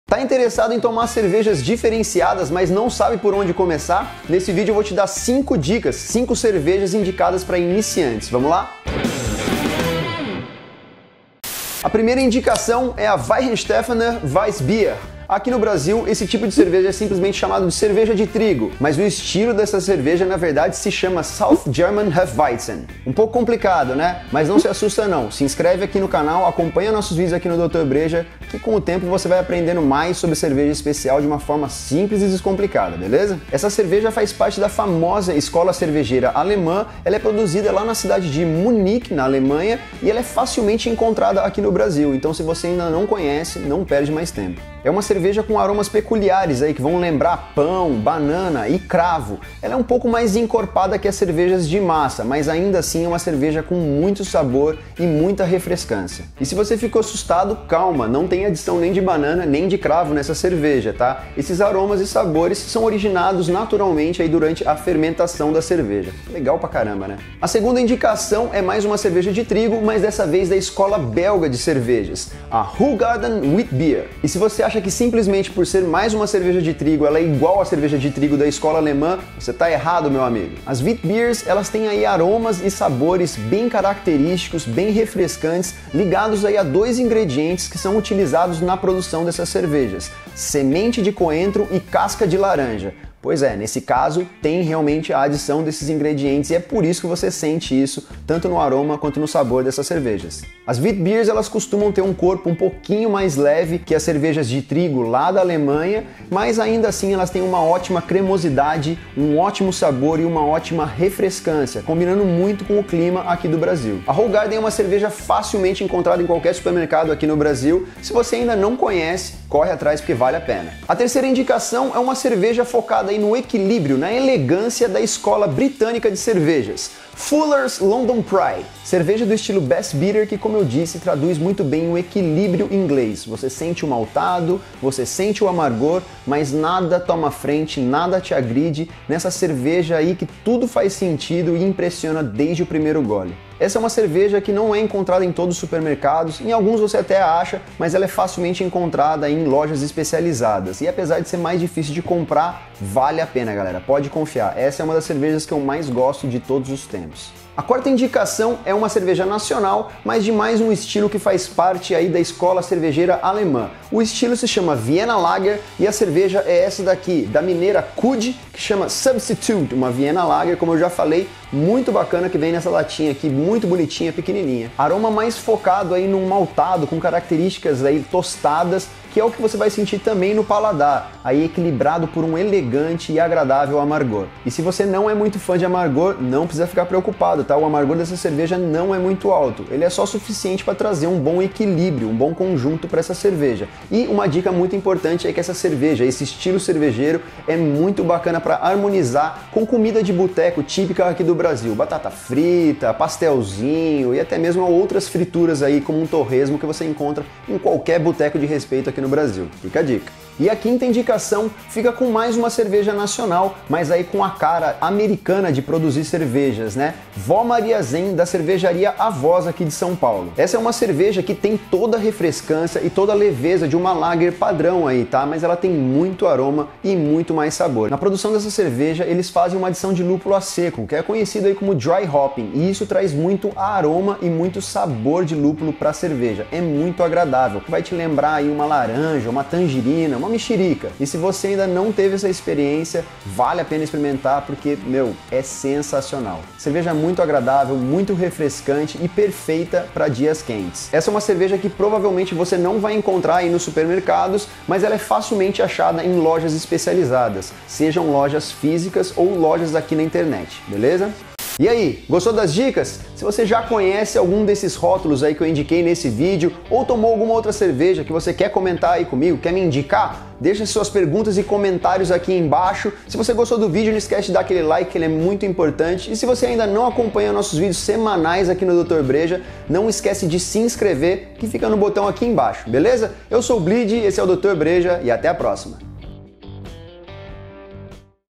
5 cervejas indicadas para começar a beber cervejas diferenciadas | DB#160
Muita gente quer começar tomar cervejas diferenciadas, mas fica perdido diante de tantas opções que encontramos no mercado hoje em dia.

Pensando em você, que está começando agora sua jornada cervejeira, gravamos esse vídeo com 5 rótulos que irão te apresentar um mundo totalmente novo, mas sem te assustar.

Tomou algum desses rótulos? Gostou? Deixe aqui embaixo seus comentários e dúvidas que teremos o prazer em te responder.

Nosso objetivo é fazer você entender mais sobre o que está bebendo, bem como aprender a beber em menores quantidades, mas com maior qualidade!